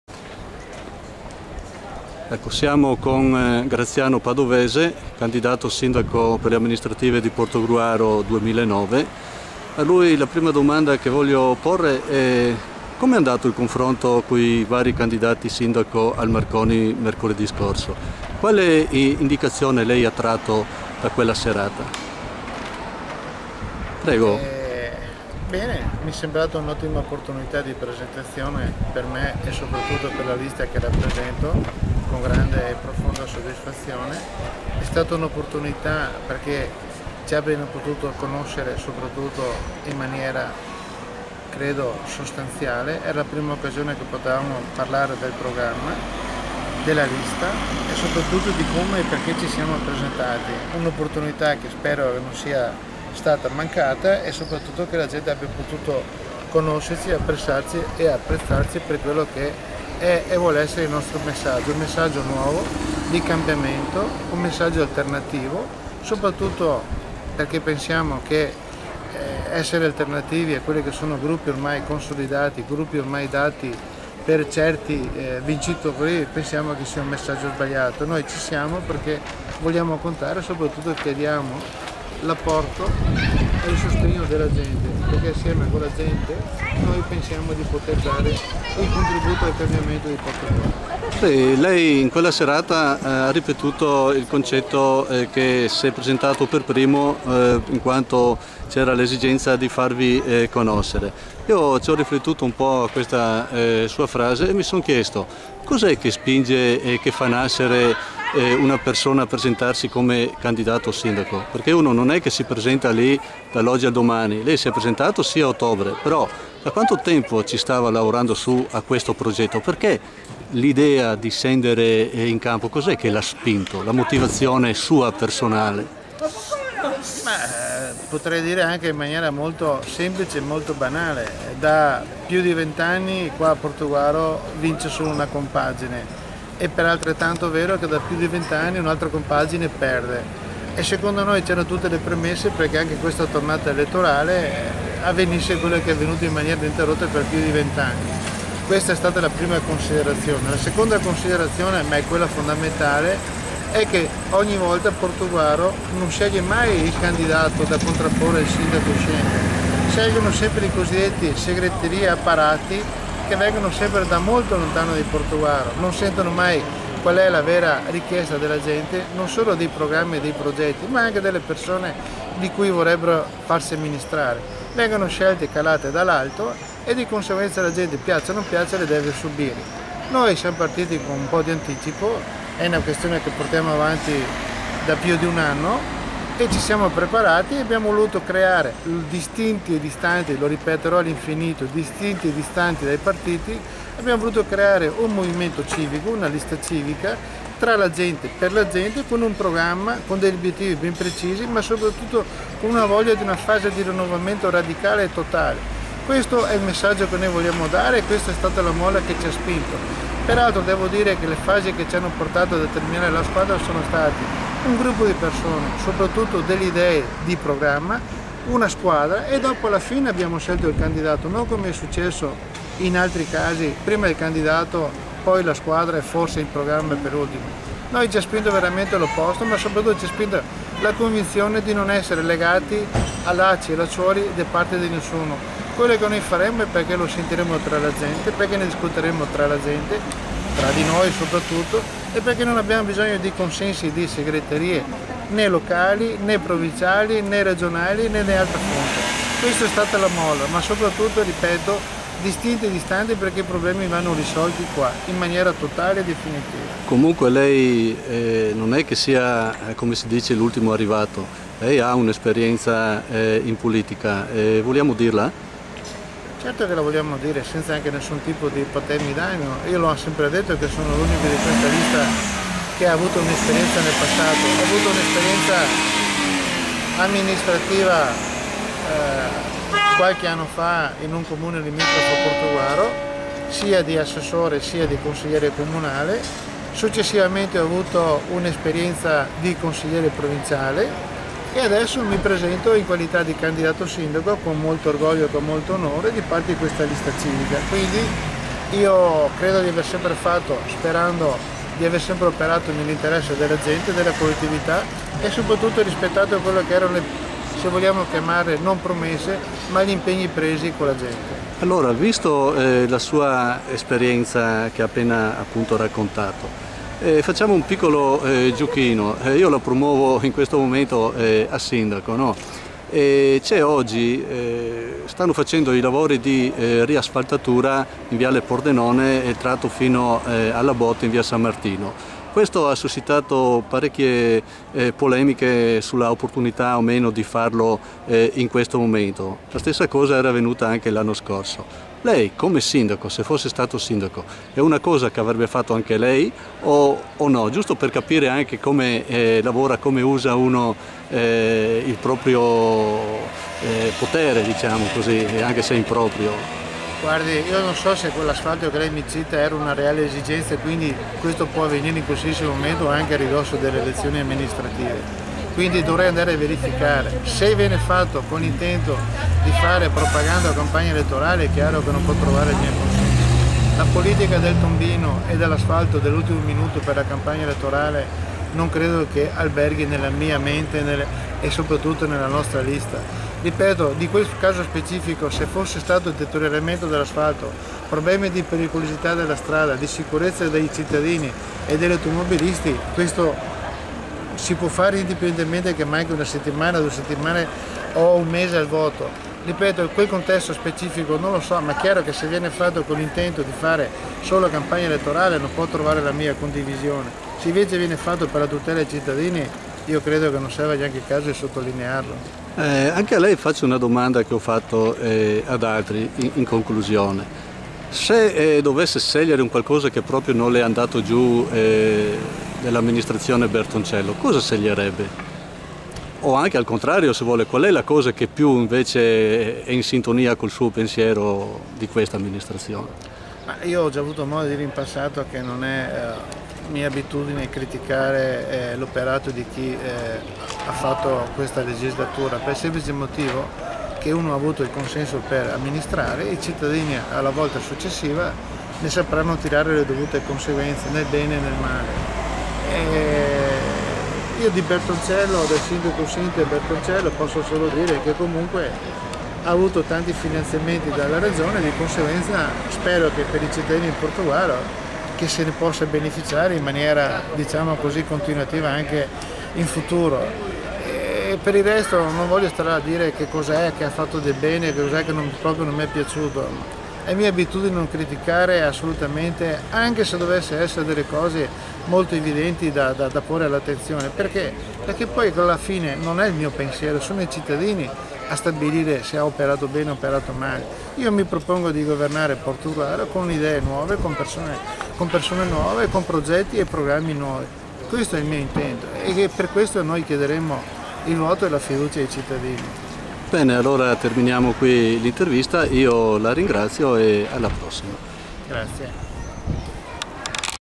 INTERVISTA GRAZIANO PADOVESE
Intervista al candidato sindaco per Portogruaro, Graziano Padovese, che si presenta con una Lista Civica...